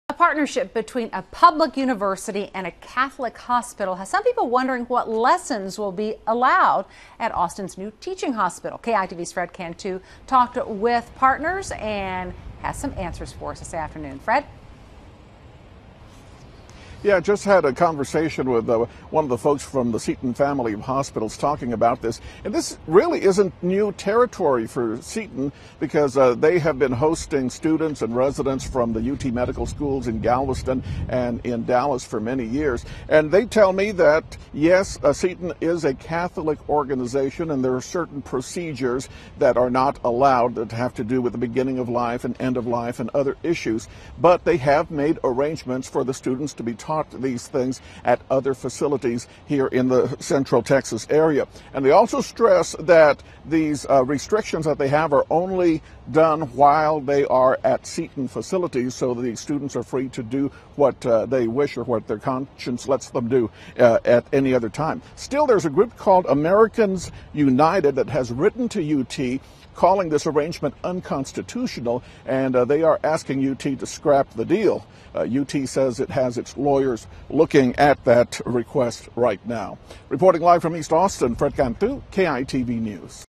Seton/UT Medical School Partnership Raises Questions
A partnership between a public university and a Catholic hospital has some people wondering what lessons will be allowed at Austin's new teaching hospital.  The Seton Healthcare Family is used to these questions regarding separation of church and state.  The Catholic-rooted organization began running Austin's city-owned hospital nearly 20 years ago.  So they're not anticipating any problems at the new Dell Medical School.The Dell Medical School will be quite a feather in the cap for the University of Texas.  But UT isn't alone in this project.  The medical school is a partnership with the Seton Healthcare Family, and that means the staff and students with have to abide by the same Catholic teachings that Seton lives by.Obvious conflicts arise when teaching students about sterilizations or pregnancy terminations.  These procedures can't be done on Seton's clock or in Seton facilities. But this is nothing new.  At publicly-owned University Medical Center Brackenridge, Seton has routinely outsourced these procedures for years.  And Seton has hosted medical students from UT Southwestern Medical in Dallas for many years.Seton is also only enforcing its doctrine on site.  Staff and students can essentially do what they want off the clock.  Still, a group called Americans United has written to UT calling their partnership with Seton unconstitutional and asking that it be scrapped.  UT lawyers are looking over that request.The new Dell Medical School is still selecting architects and contractors.  it's not expected to begin operation until 2016.By Fred Cantu